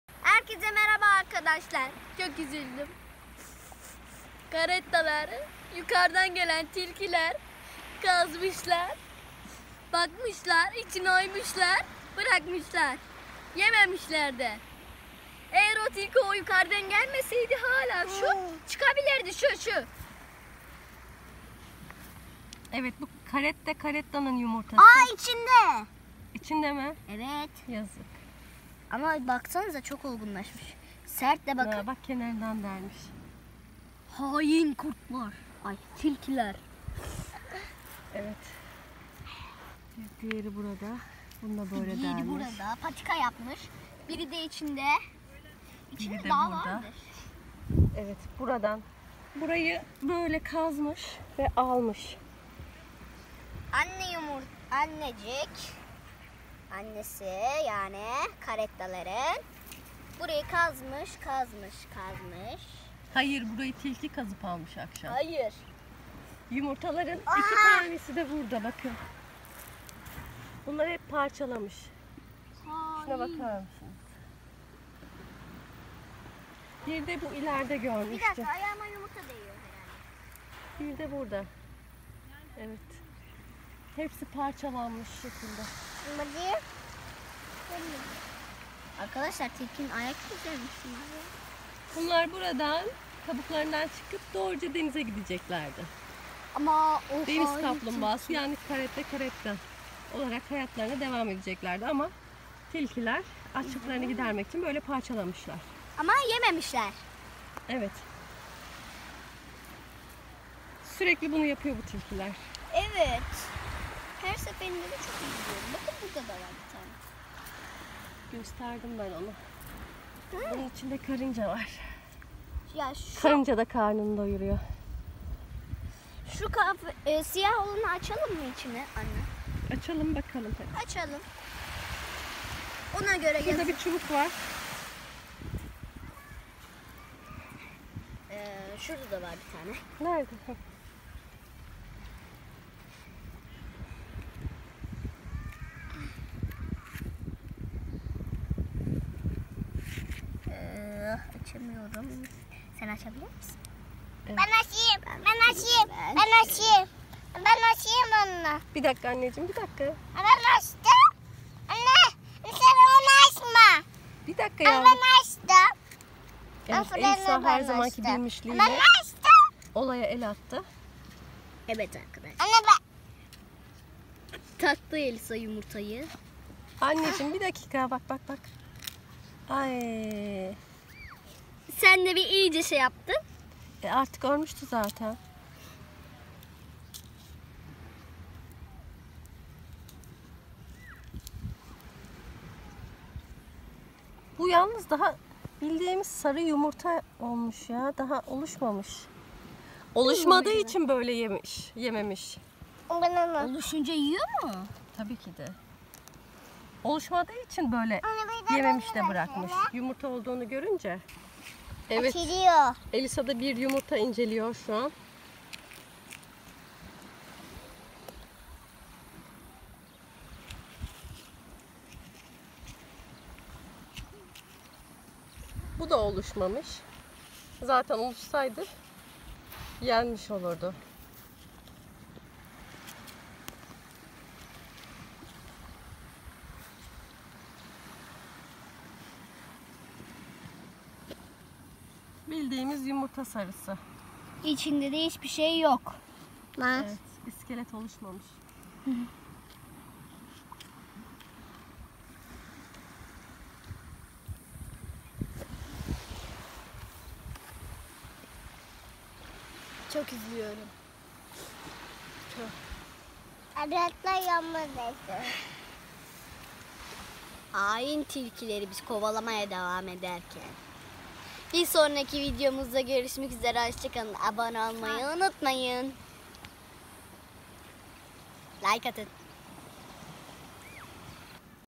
CARETTA CARETTA ( DENİZ KAPLUMBAĞASI ) YUMURTALARININ BAŞLARINA GELENLER EĞLENCELİ ÇOCUK VİDEOSU
Merhaba arkadaşlar. Bugün kumsalda Deniz kaplumbağası yani caretta caretta yumurtalarını gördük ve bunları sizler için inceledik ve üzücü görüntülerle karşılaştık. Çünkü bu yumurtaları gece tilkiler kumsala inerek parçalamış ve bu yumurtalar dünyaya gözlerini açıp denizlerde yüzememişlerdi. Keşke bu yumurtalar için en kısa zamanda koruma alanı yapılsa da nesli tükenmekte olan caretta yumurtaları denizlerde yüzebilse.  Hep birlikte bu çocuk videomuzu izleyelim.

Minti TV kanalımızda eğlenceli videolar ile sizlere güzel vakit geçirmek istiyoruz.